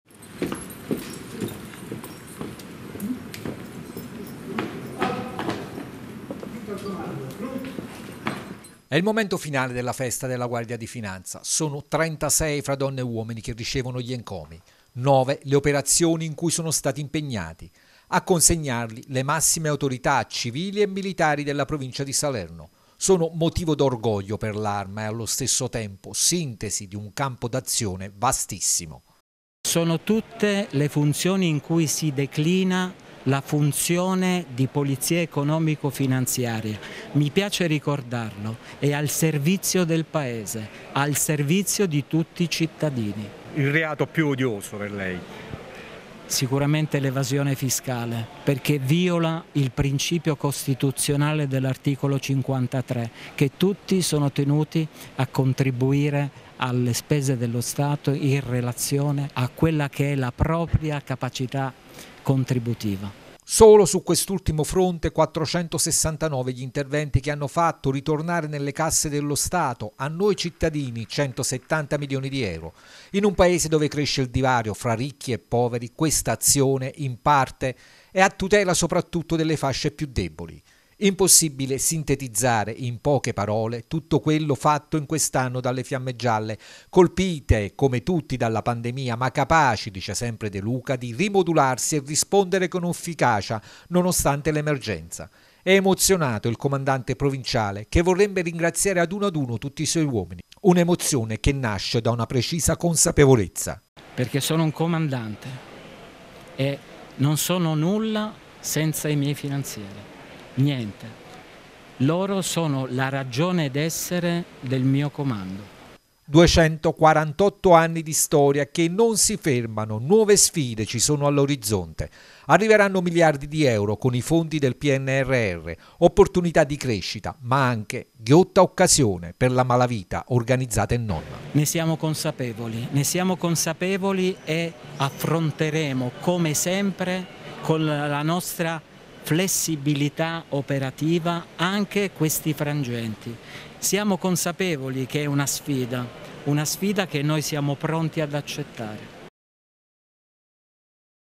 Guardia di Finanza, festa a Salerno per i 248 anni di vita, "Siamo al servizio paese e cittadini"
Guardia di Finanza, festa a Salerno per i 248 anni di vita, "Siamo al servizio del paese, di tutti i cittadini" dice comandante provinciale dell'arma il generale Oriol de Luca